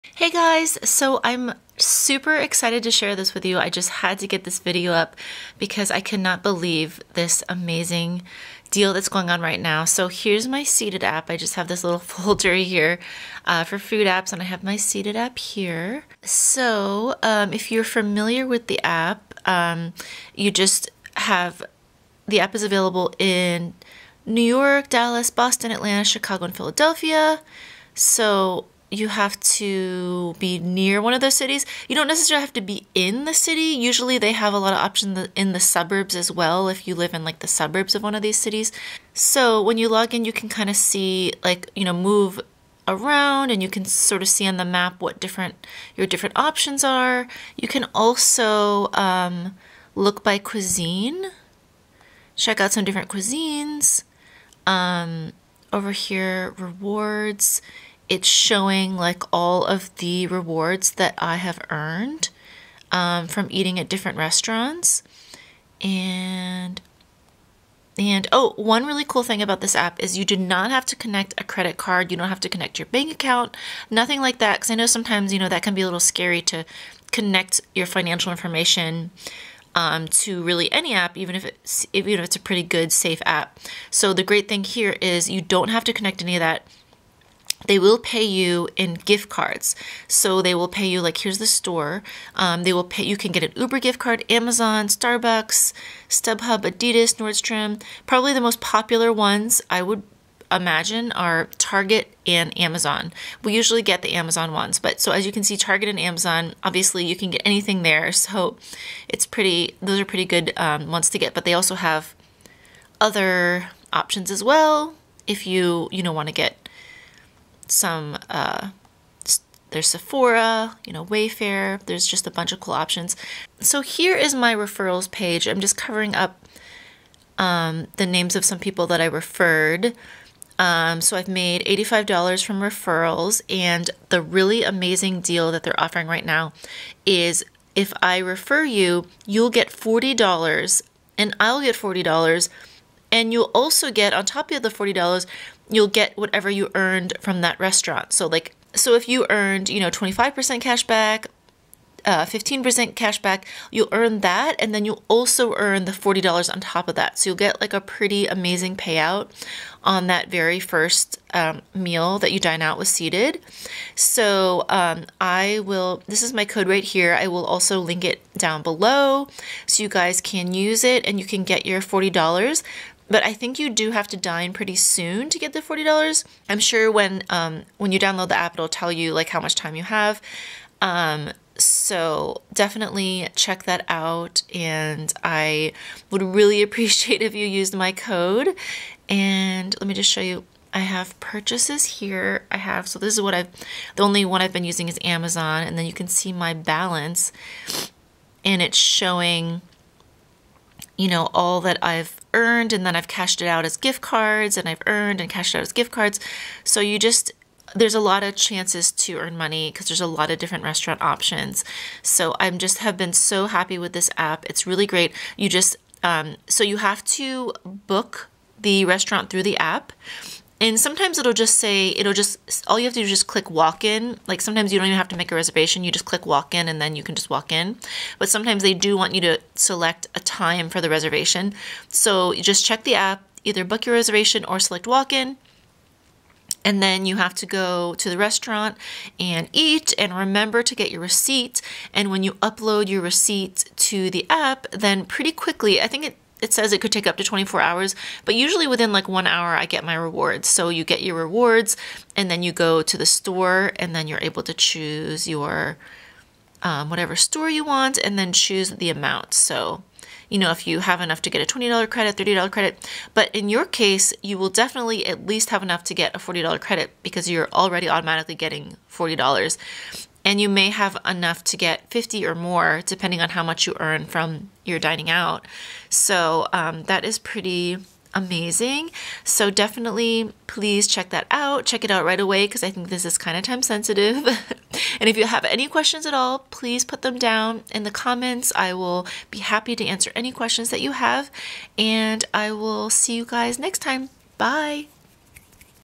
♥ ♥GET $40 BACK WHEN YOU DINE OUT!! SEATED APP REVIEW♥ ♥
You can get the Seated app on Apple or Android phones.

These are the seated cities, and they offer restaurants in the suburbs of these cities as well:
Atlanta
Boston
Chicago
Dallas
New York City
Philadelphia

Get a $40 bonus from your first purchase through Rakuten! They've got Sephora, Ulta, Target, the list goes on!